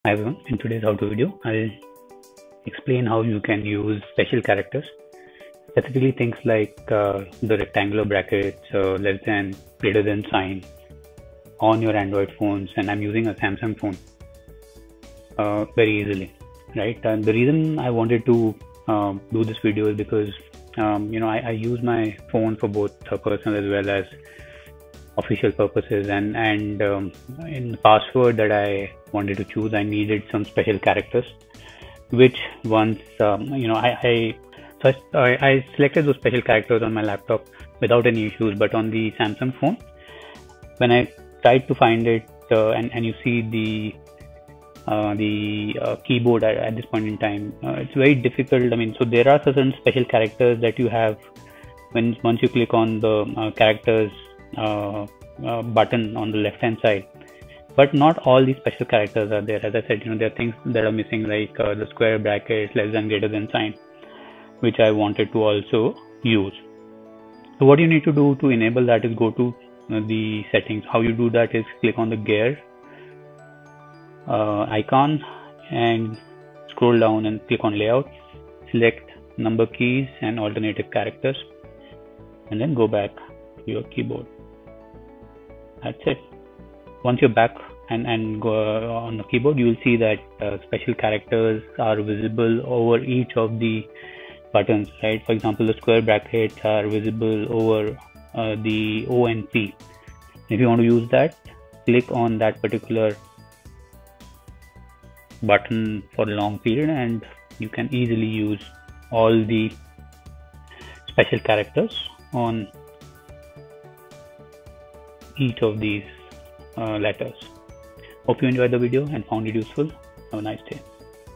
How to use special characters on your Android phone (2022)
In this video I demostrate how you can use special characters on your Android phones.

This is especially useful when you are using special characters in your passwords.

I am using a Samsung phone.